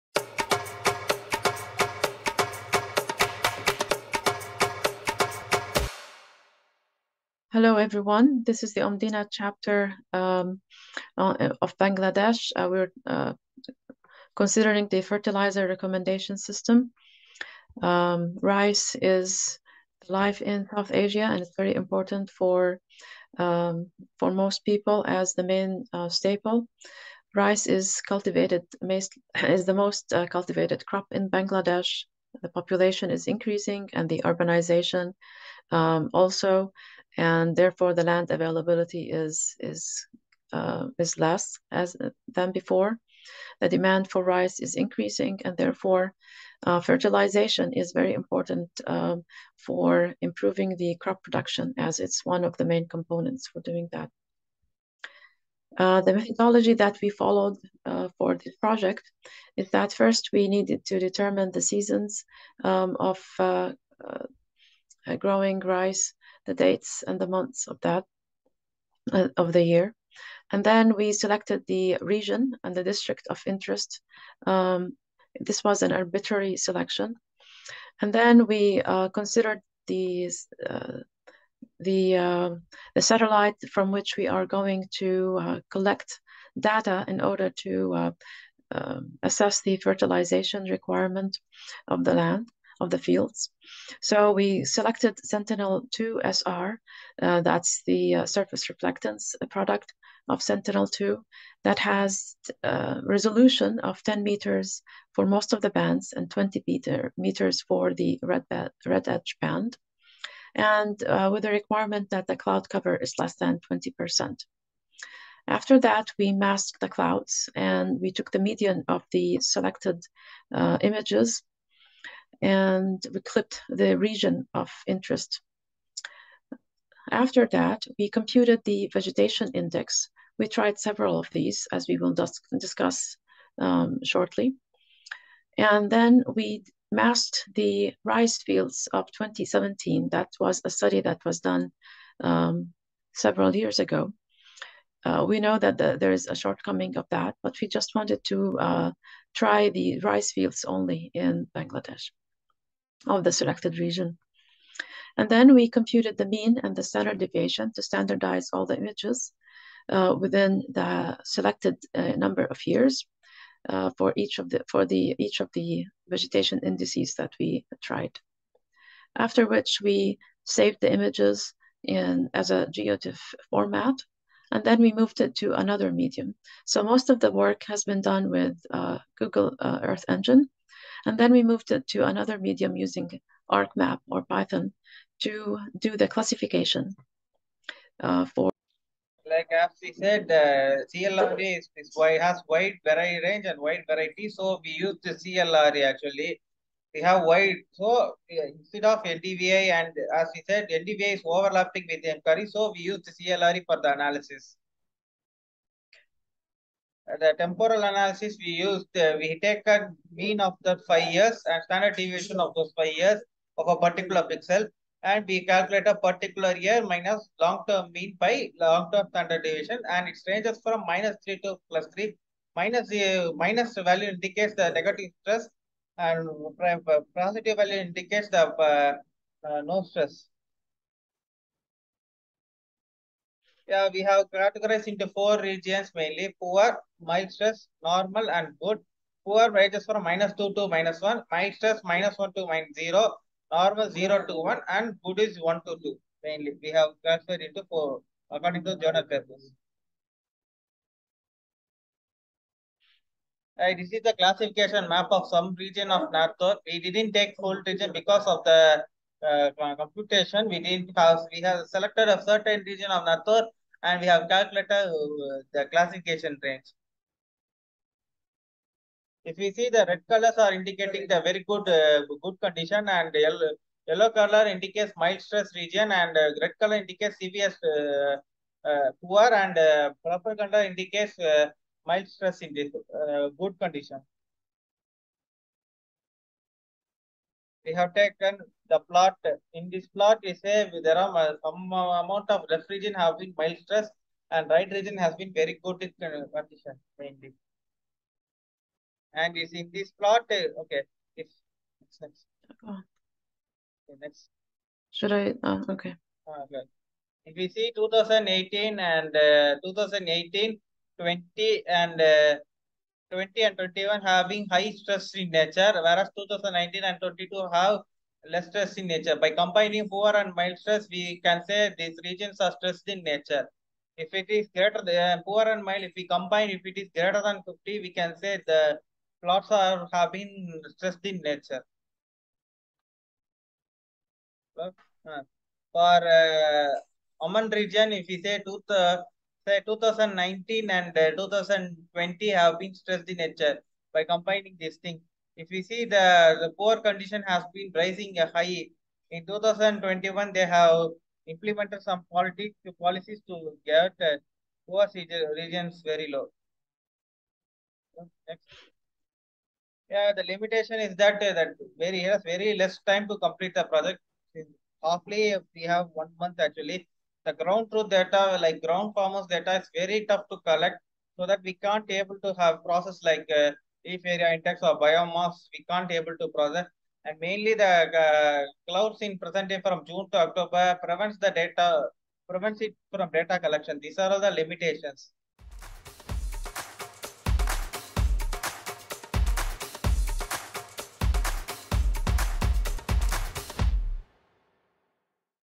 A Fertilizer Recommendation System for Higher Crop Yields in Bangladesh
This project stems from the Omdena Bangladesh Local Chapter led by Saiful Islam Shawon and Fahad Munir

Collaborators:

Hala Jadallah
Sai Villiers
Bakhtiyar Babashli
Md Hossen Sohrawardy
MUSTAPHA MOSHOOD
Fahim Muntasir
Jawaher Akhter